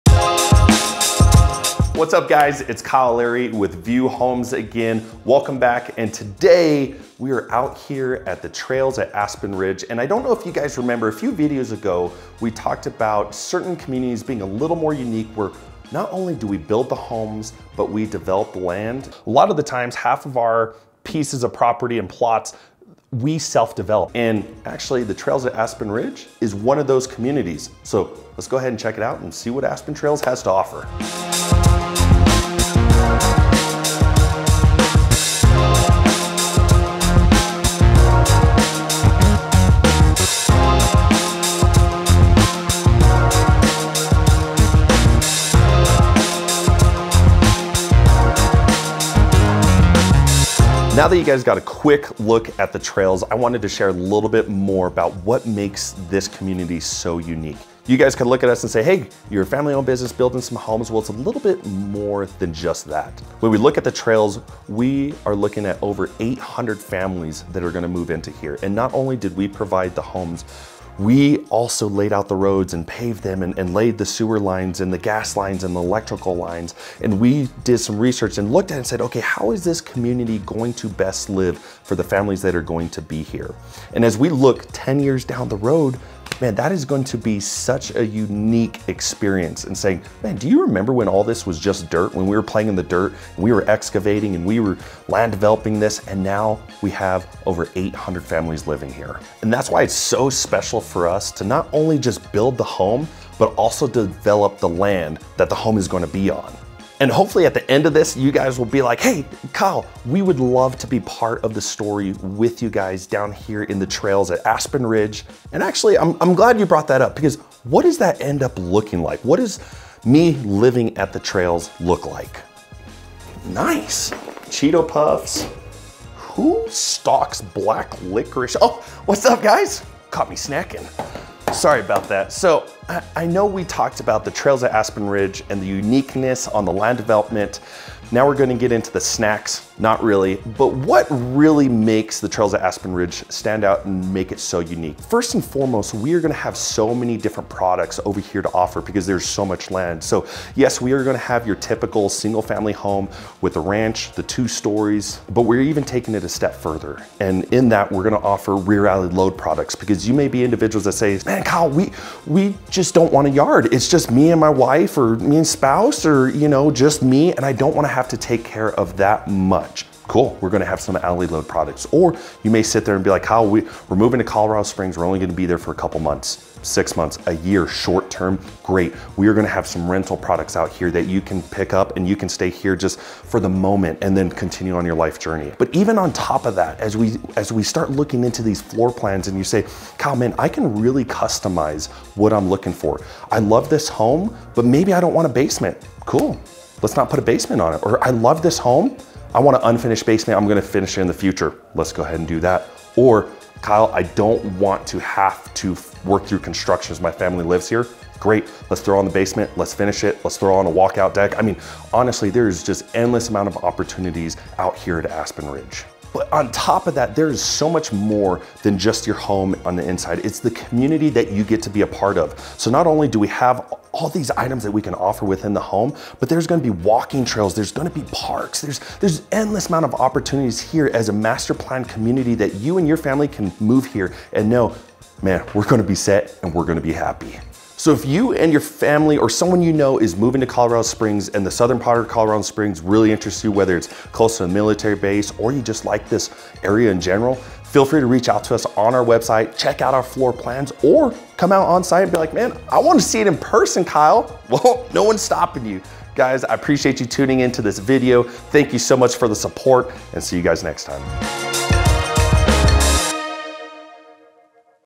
The Trails Community in Colorado Springs
Welcome to one of our beautiful communities, in Colorado Springs, THE TRAILS. In this video we give you an overview of everything you can expect when you build in this community... ENJOY!